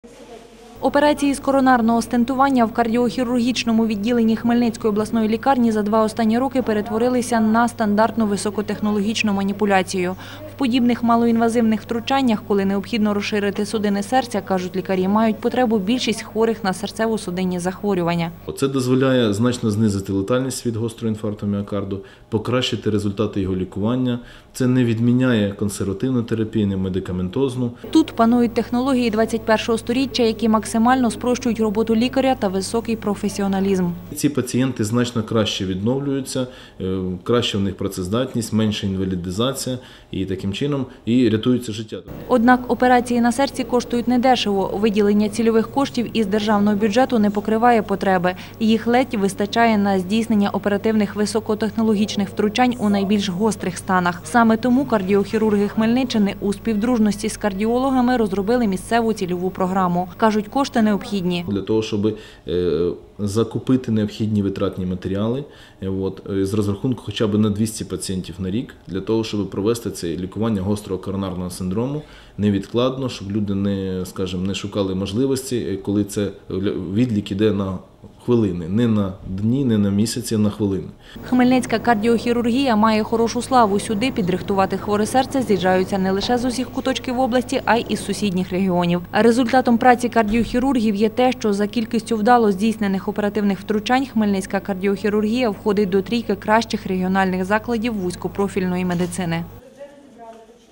Кардіохірургія
Чи готові подільські медики до боротьби з хворобами серцево-судинної системи;
Автор: А.Бендюг